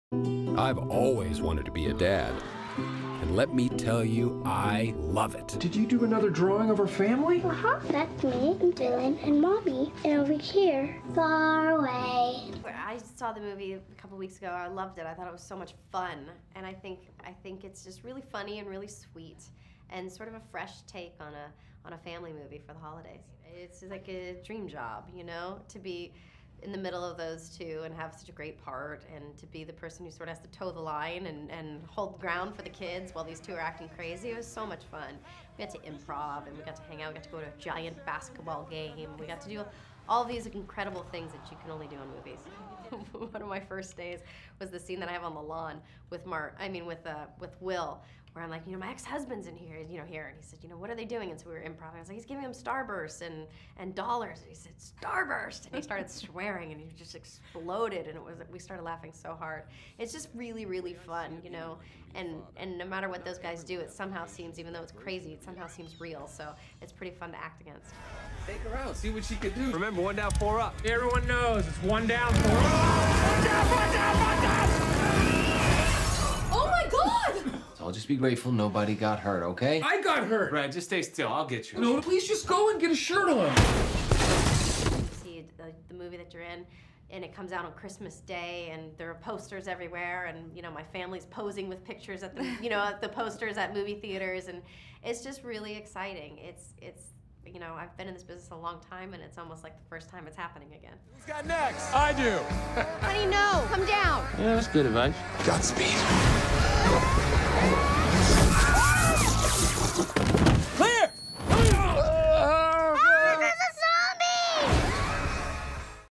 Linda Cardellini from "Daddy's Home" Interview
Actress Linda Cardellini discusses her role in "Daddy's Home"